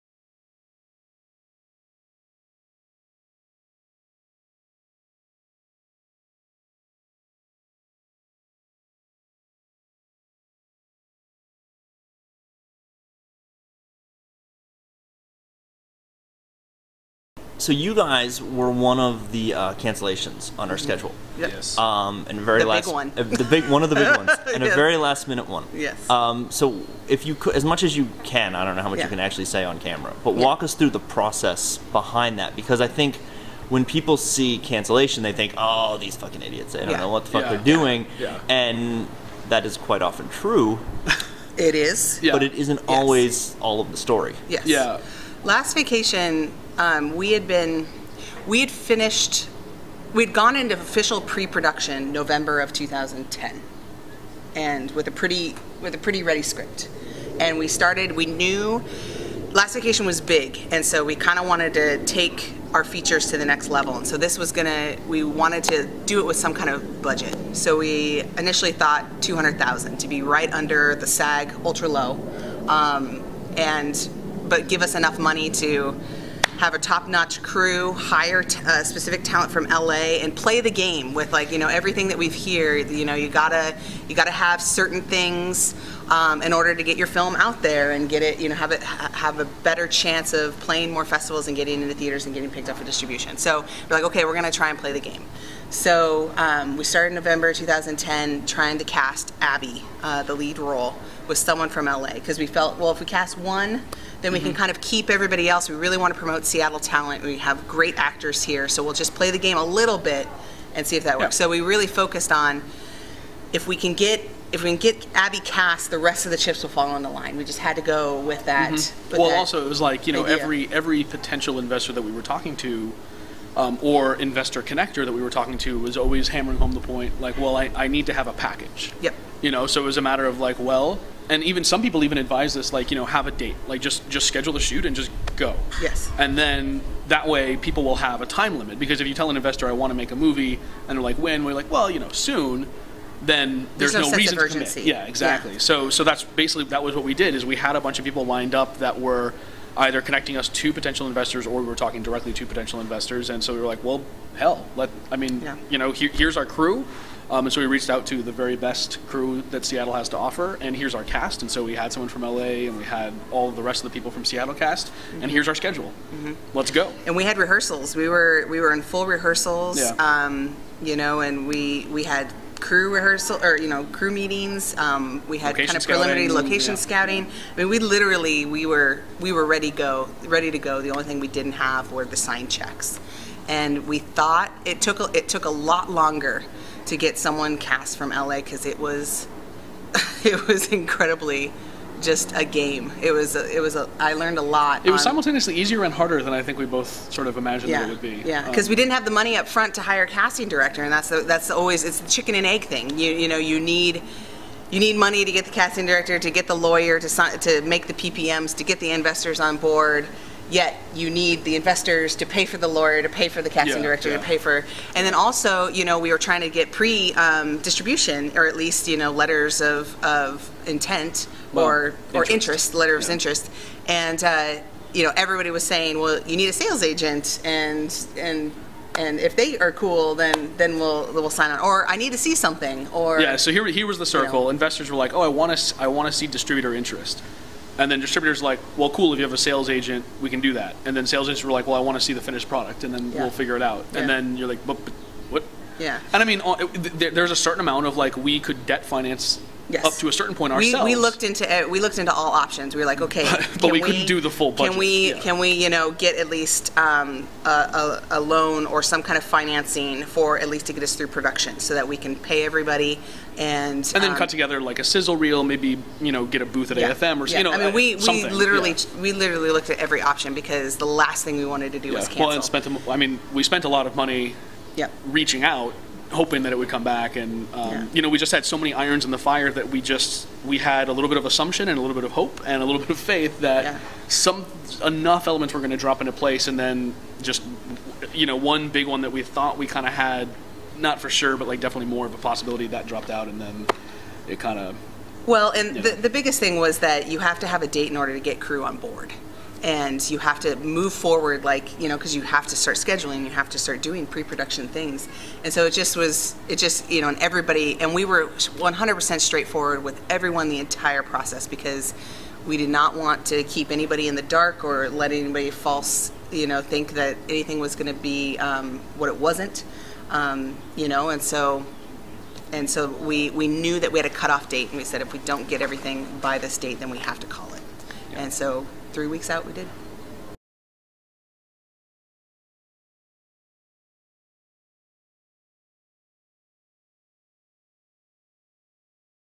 Conversations with Lucas: Kris Lindy Boustedt (part 3)
Remember all of those cancellations? The ones that really screwed up our schedule? Well, I tracked down one of them. ;